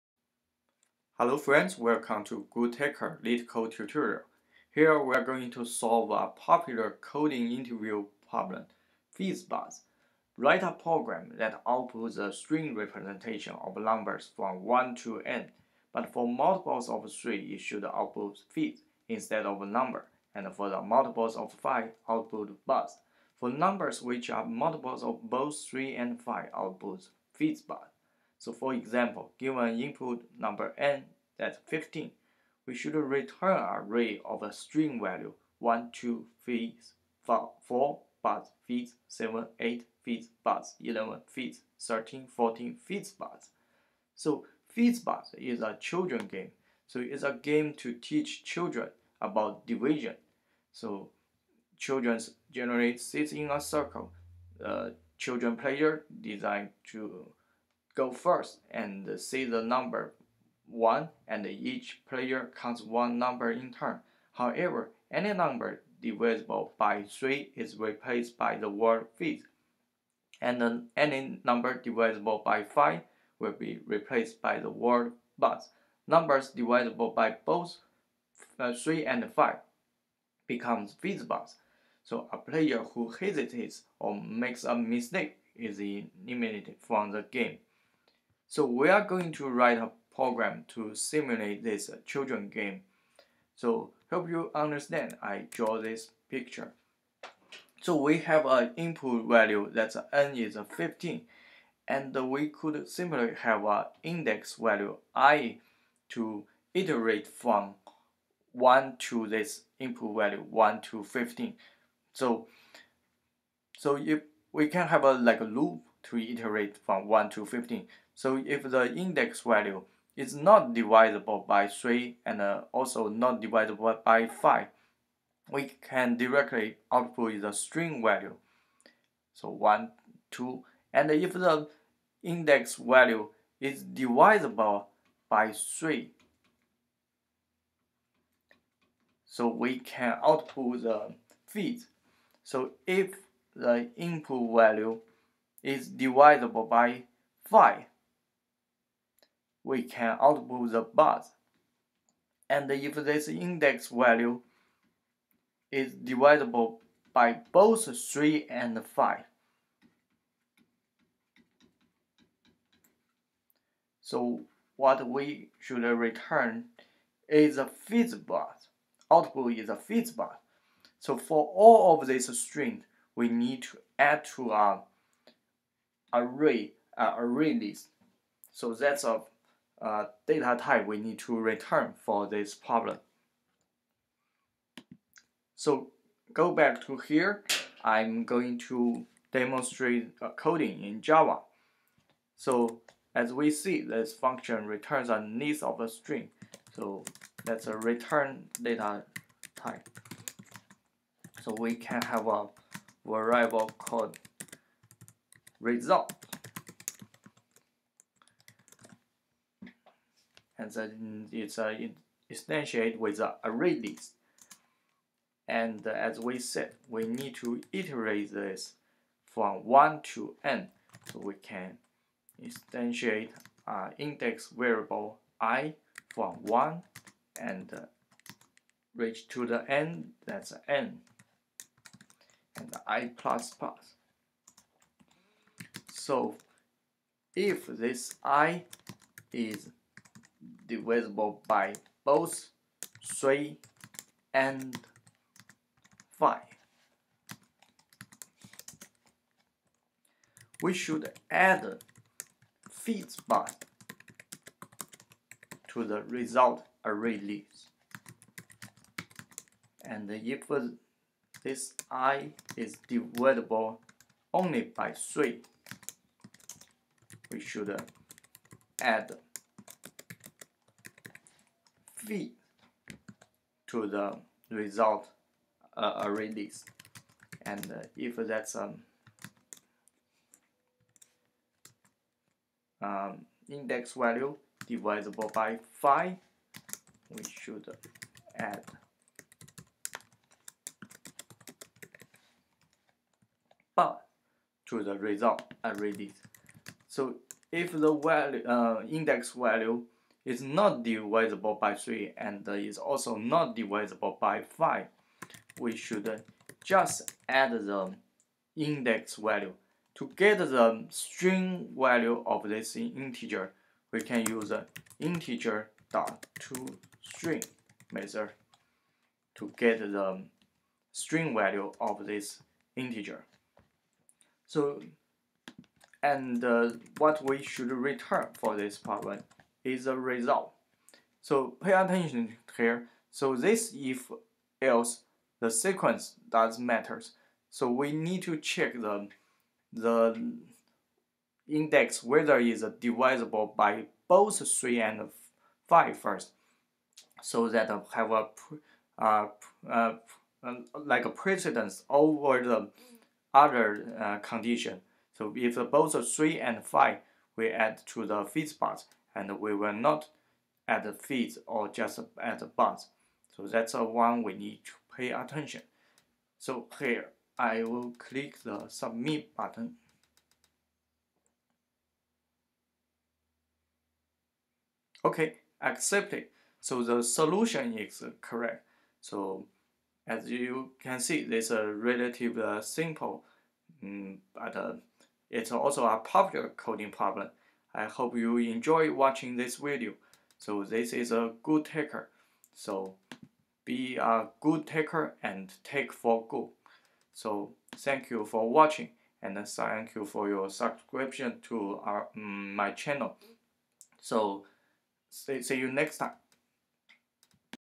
LeetCode Tutorial 412. Fizz Buzz
GoodTecher LeetCode Tutorial 412. Fizz Buzz (Java)

LeetCode Tutorial by GoodTecher. The tutorial provides a step-by-step demonstration on how to solve coding problems.